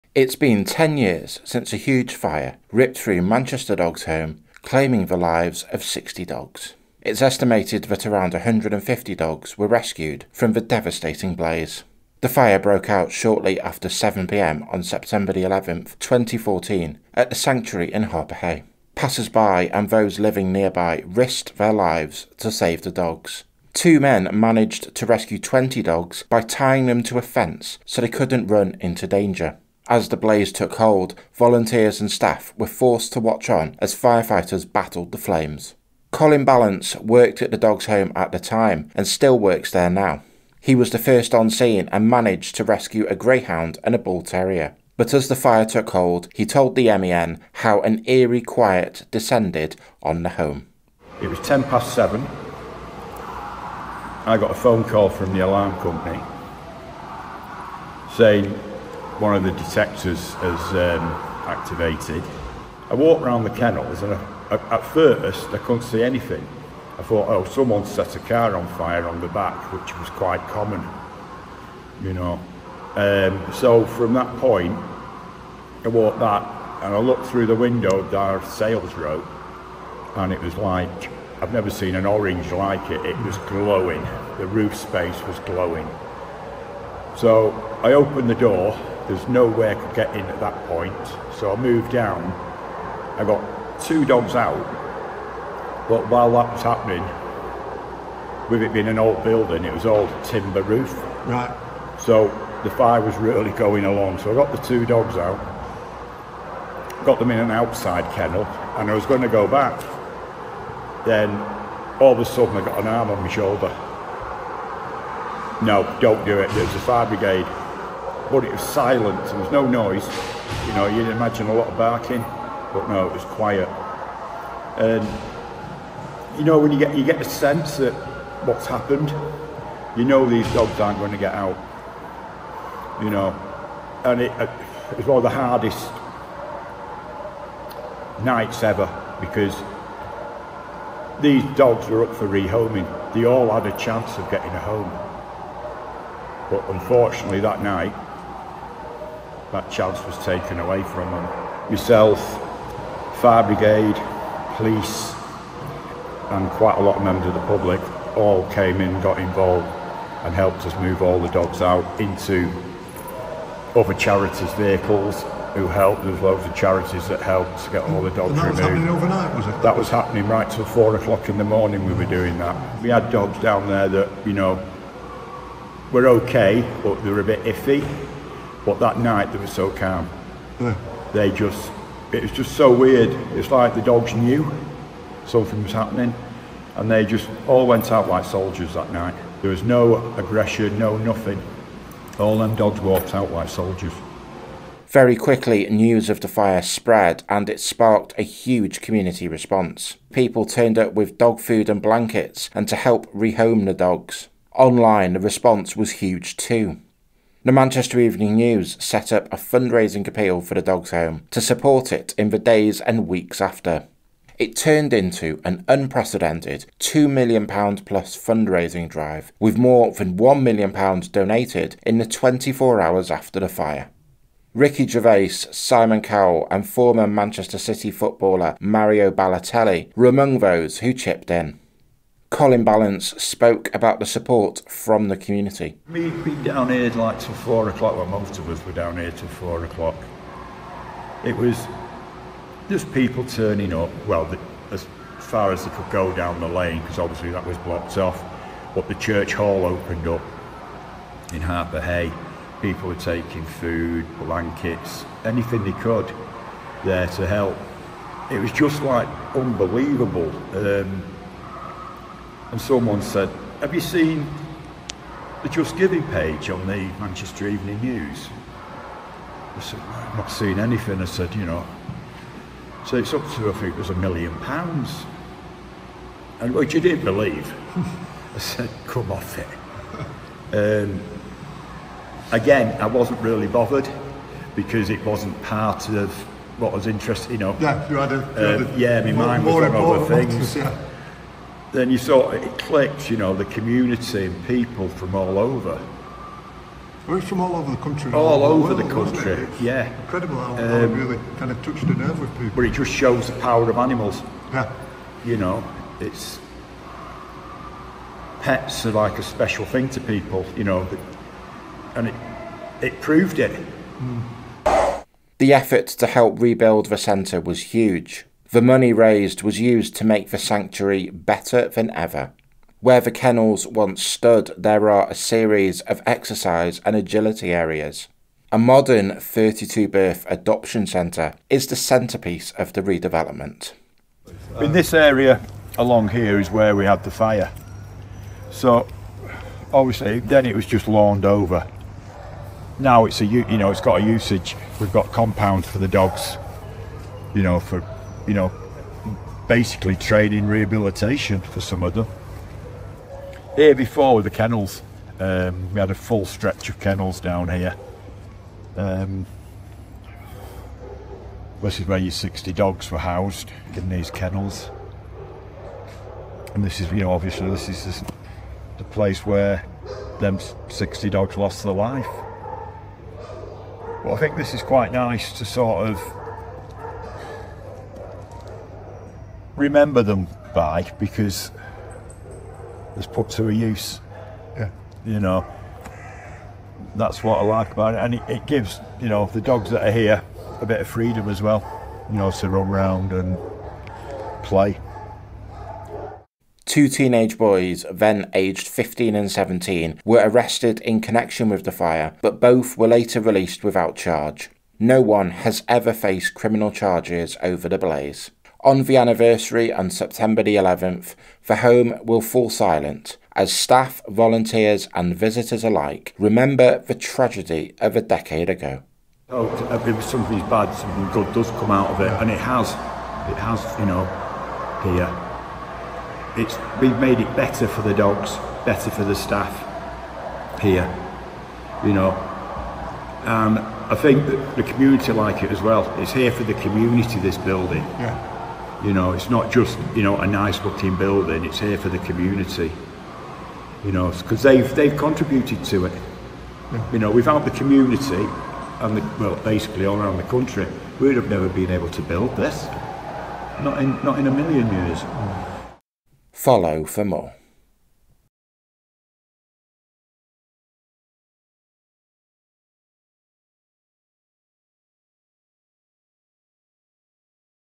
The Manchester Dogs' Home fire that claimed the lives of 60 dogs
It has been 10 years since the Manchester Dogs' Home fire.

The blaze - which broke out shortly after 7pm on September 11, 2014 - claimed the lives of 60 dogs and an estimated 150 were rescued from the building.

In the hours, days and weeks that followed, a huge fundraising effort saw more than £1m raised for the shelter and an overwhelming effort to help - with people rushing to rehome the dogs, bring shelter and food, and do whatever they can.

Today (September 11), the home will fall silent, as staff, volunteers and visitors alike remember the events of a decade ago.